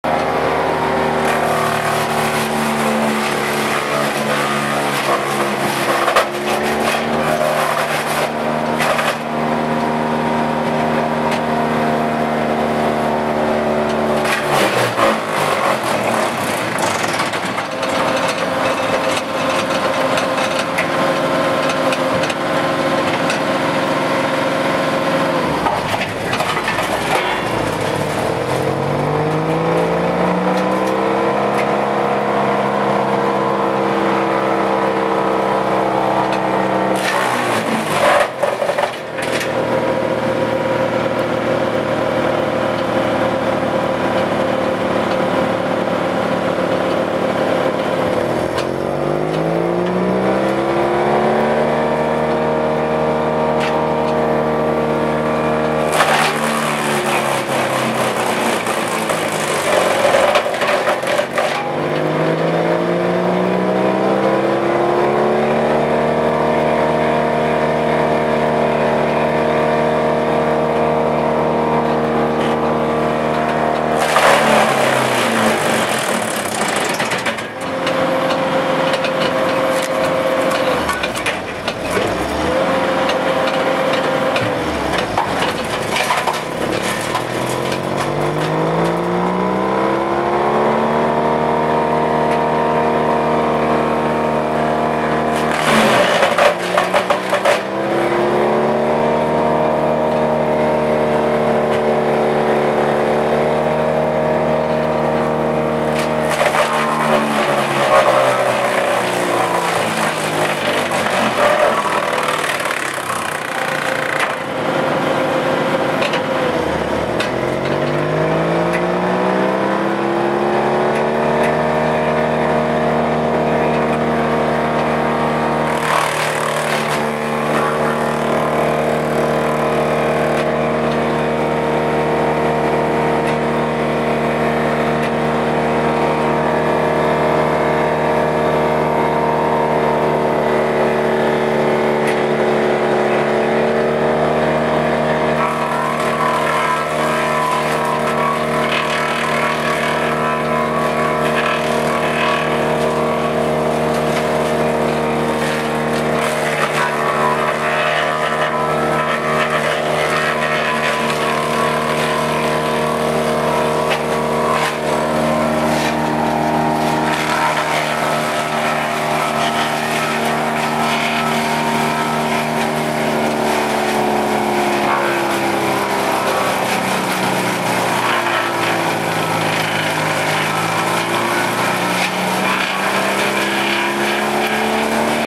Chipeadora Morbark M12R-3-
Chipeadora de ramas Morbark con capacidad hasta 30cm de diametro. El equipo esta montado sobre trailer y motor caterpillar de 84HP.
Contamos con la linea completa de chipeadoras desde equipos con capacidad de chipear madera de 15cm hasta equipos de arboles completos con motores de 1000hp.